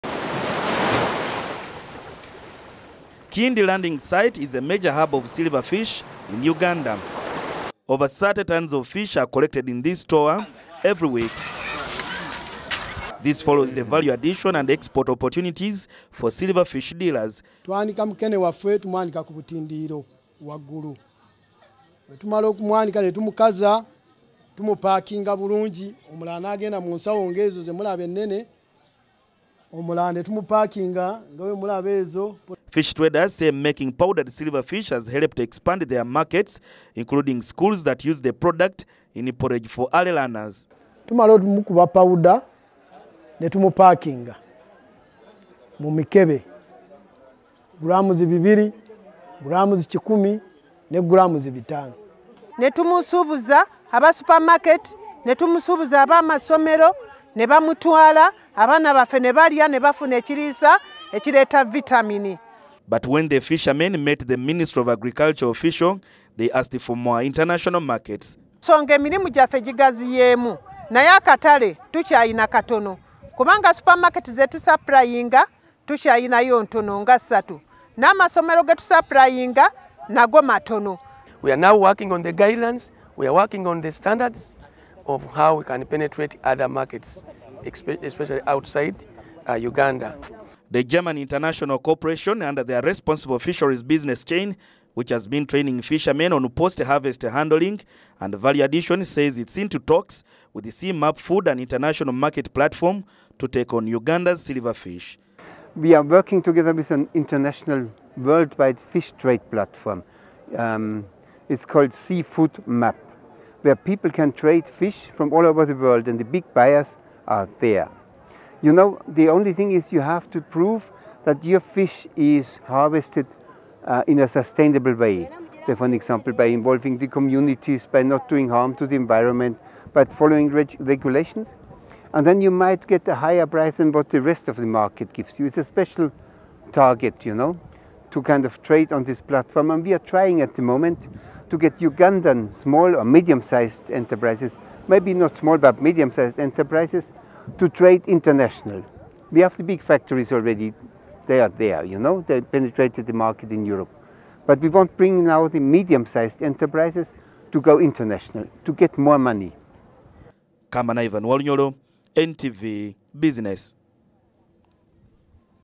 FISH EXPORTS: EU targets silver fish
Silverfish may be the newest addition for Uganda on the list of fish exported to Europe. This is part of efforts by government and development partners at improving the silverfish value chain.

To this end, the campaign began with training to sector stakeholders by the German development agency, an initiative aimed at expanding Uganda’s export offerings in the fish sector through one of the EU's biggest fish platforms.